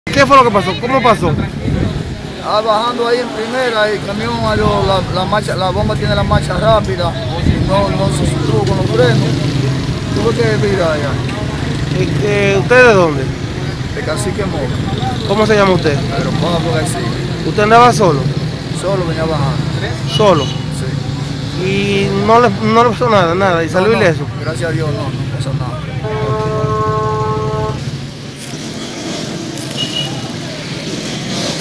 Camión cargado de plátanos se vuelca en carretera Tenares-Salcedo
Un camión cargado de plátanos terminó volcado al mediodía de este lunes, próximo a la entrada de Jayabo, jurisdicción del municipio de Salcedo, luego de que se saliera de la vía, al parecer, motivado a un desperfecto en los frenos.

El nombrado Pedro Pablo García, residente en la comunidad El Cacique de Moca, quien conducía el vehículo dijo, que el hecho ocurrió cuando intentó reducir la velocidad de dicho camión y los frenos no le respondieron, por lo que se vio en la obligación de estrellarlo en la vía.

En dicho accidente no hubo perdida humana, solo material.